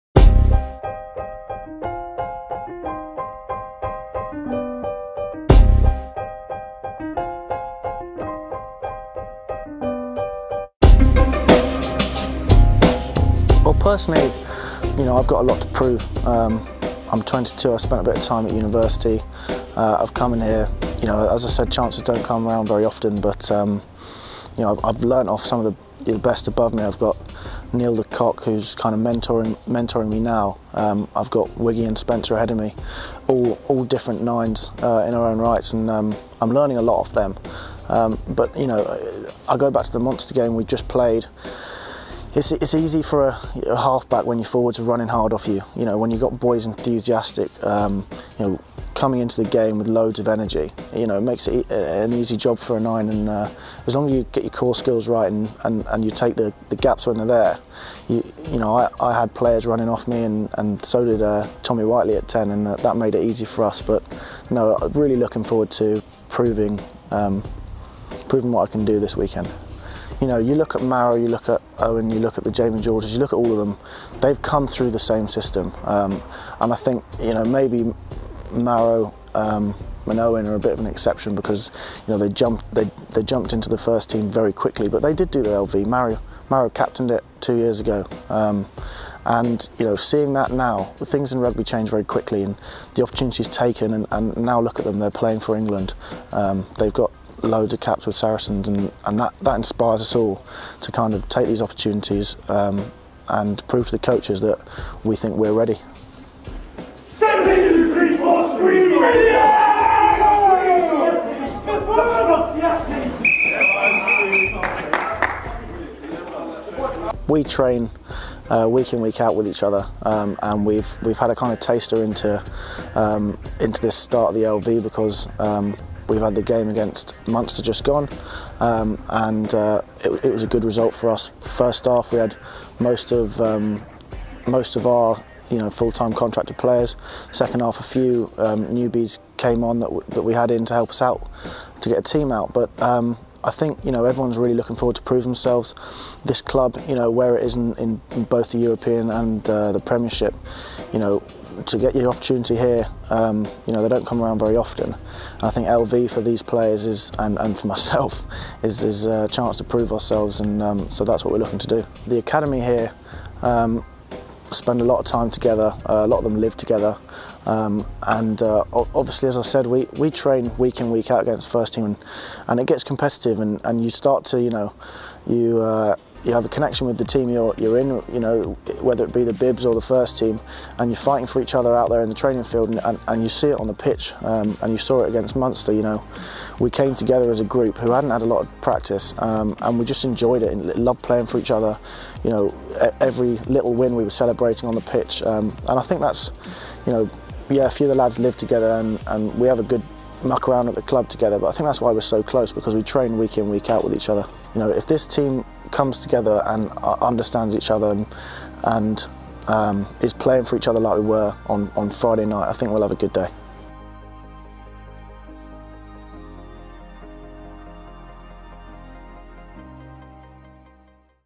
Sarries Insights | "I've got a lot to prove" - Henry Taylor
Scrum half Henry Taylor features in this week's Sarries Insights